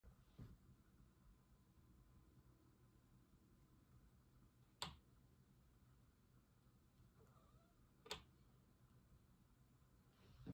When the drawer is not quite open enough. rescuecats tabbycat #
Stormy’s little meow at the end is amusing.